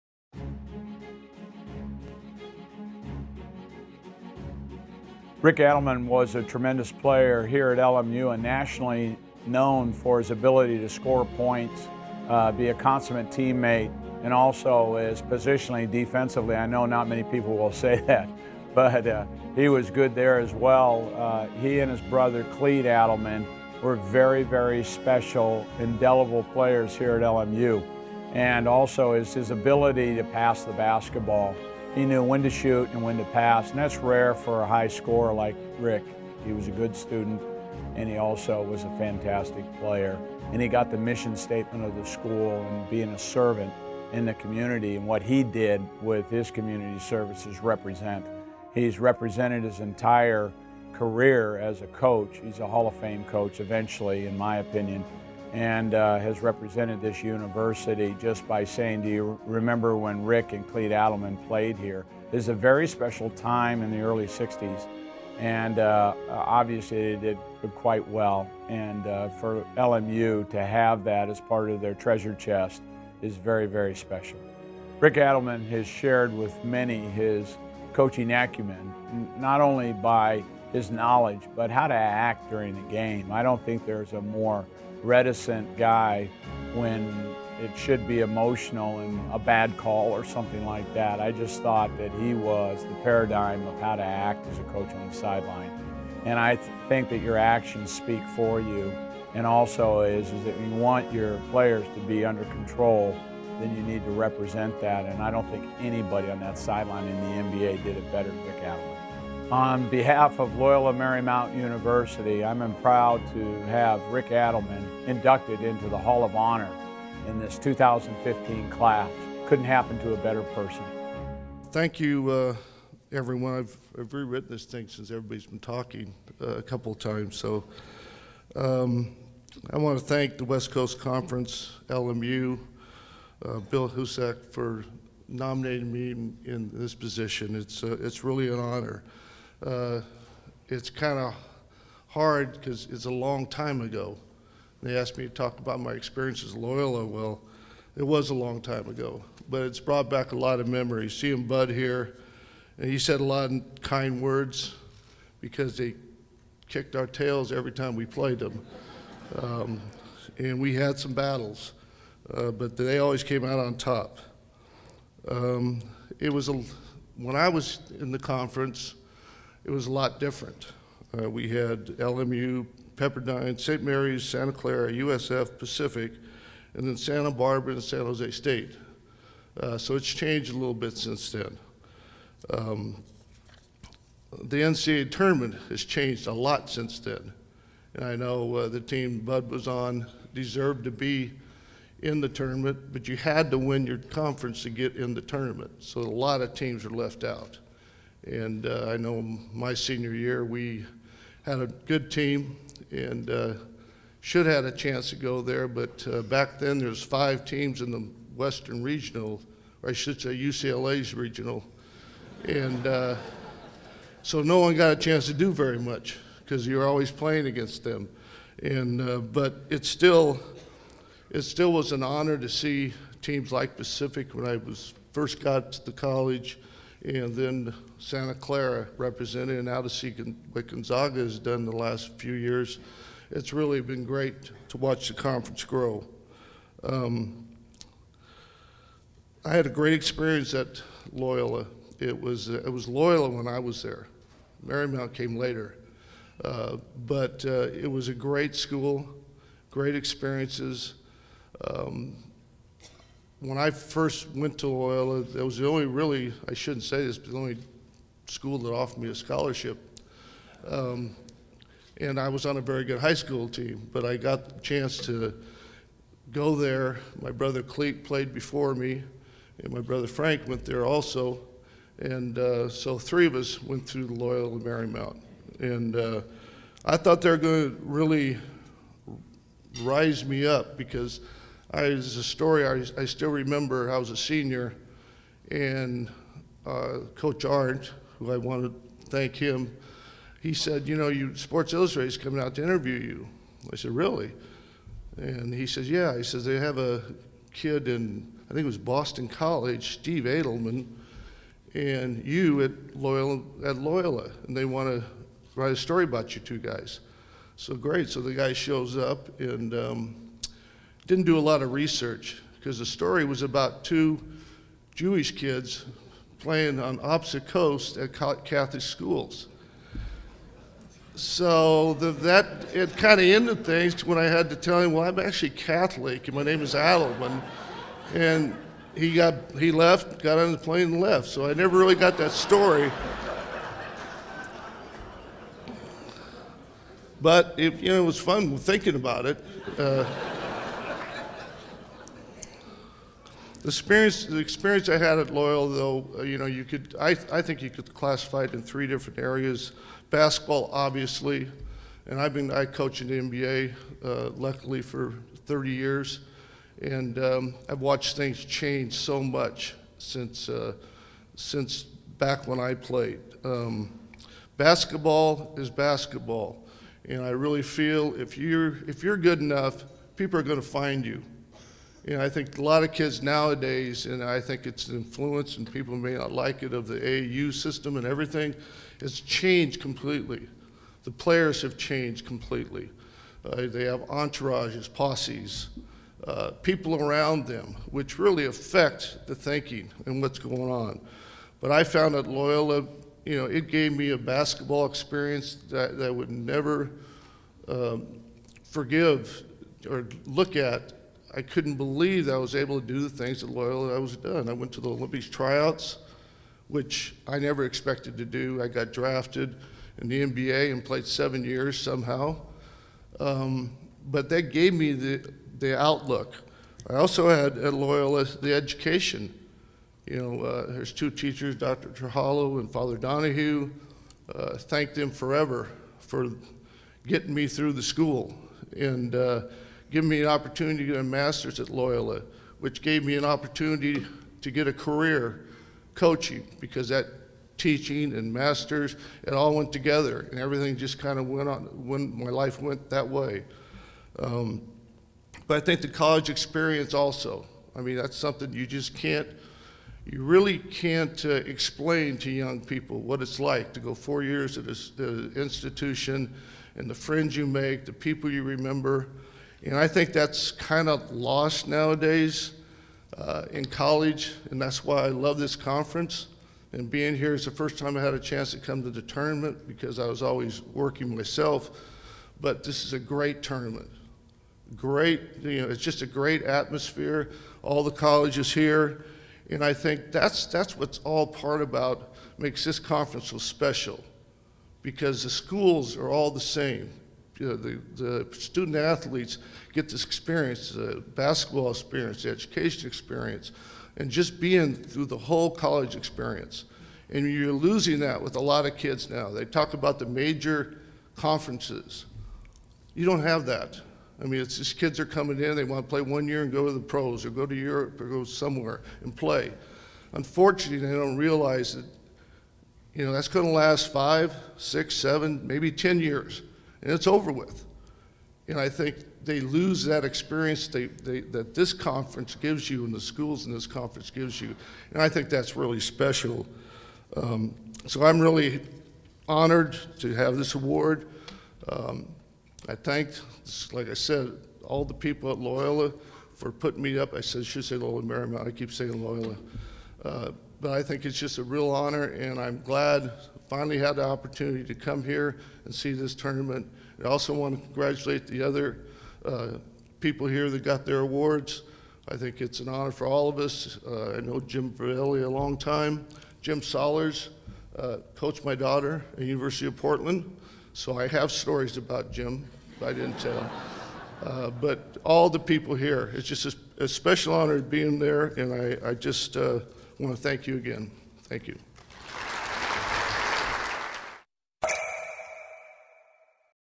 WCC Hall of Honor | LMU's Rick Adelman
Former Loyola Marymount player and NBA coach Rick Adelman was inducted into the West Coast Conference Hall of Honor on Saturday, March 7, 2015, at the Orleans Hotel in Las Vegas, NV. Watch his induction video and his acceptance speech here.